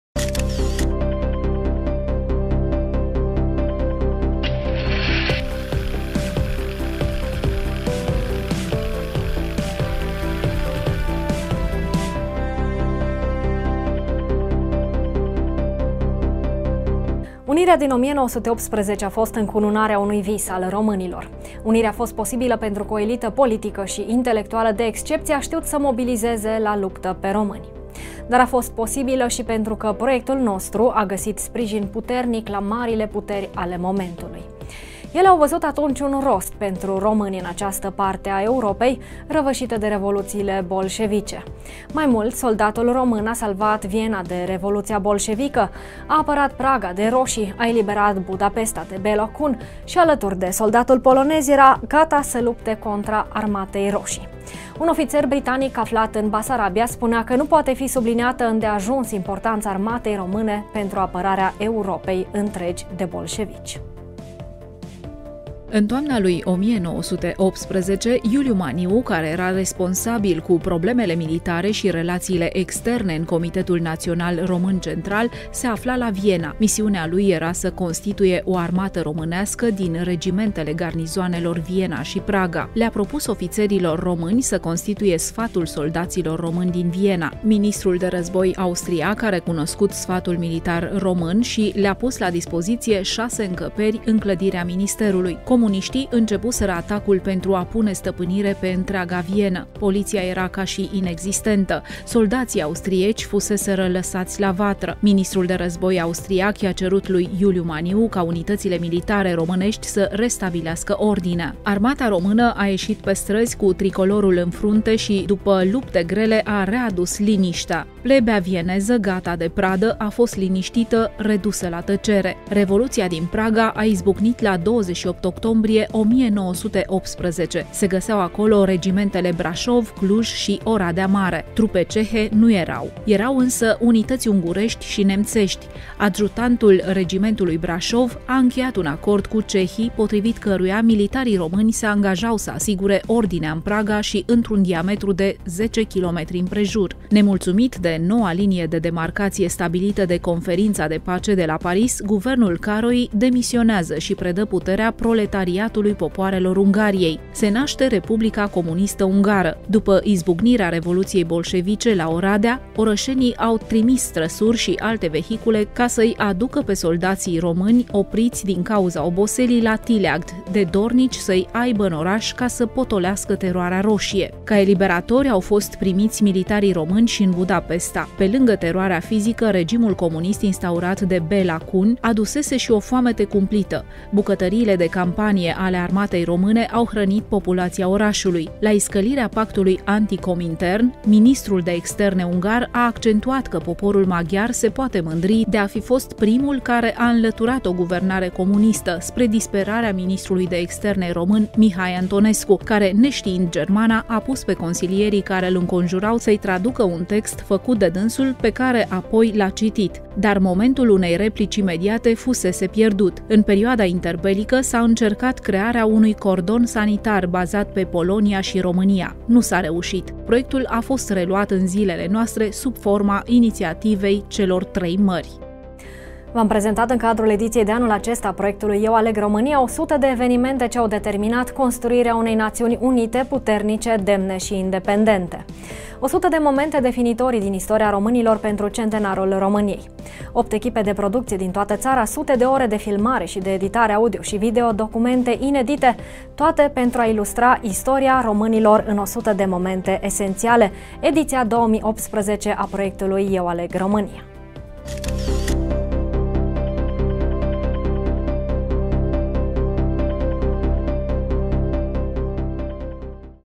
Salvarea Vienei, Pragăi şi a Budapestei de revoluţiile bolşevice
Unirea din 1918 a fost încununarea unui vis al românilor. S-a înfăptuit într-un context surprinzător de favorabil nouă, după ce în perioada anterioară am fost de mai multe ori pe punctul să pierdem şi statul care ne mai rămăsese neocupat de Puterile Centrale. Unirea a fost posibilă pentru că o elită politică şi intelectuală de excepţie a ştiut să mobilizeze la luptă pe români. Dar Unirea din 1918 a fost posibilă şi pentru că proiectul acesta al nostru a găsit sprijin puternic la marile puteri ale momentului. Ele au văzut atunci un rost pentru români în această parte a Europei, răvăşită de revoluţiile bolşevice. Au văzut că soldatul român, spre deosebire de cel rus, cel ungur sau austriac, de cel german nu a plecat urechea la chemarea revoluţiei bolşevice. Mai mult, soldatul român a salvat Viena de revoluţia bolşevică, a apărat Praga de „roşii“, a eliberat Budapesta de Béla Kun şi, alături de soldatul polonez, era gata să lupte contra Armatei Roşii. Un ofiţer britanic aflat în Basarabia în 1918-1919 spunea că nu poate fi subliniată îndeajuns importanţa armatei române pentru apărarea Europei întregi de bolșevici. El remarca excepționalul moral al soldatului român, care făcea ca încercările bolșevice de a recruta simpatizanți să fie fără sorți de izbândă.

Consultant științific – Dorin Matei, redactor-șef Magazin istoric

Radio România Tg. Mureș
Prezentare – Emanuela Aranyos
Operator video/editor video – Alin Mariș
Coordonator proiect, voice over – Nicoleta Ghișa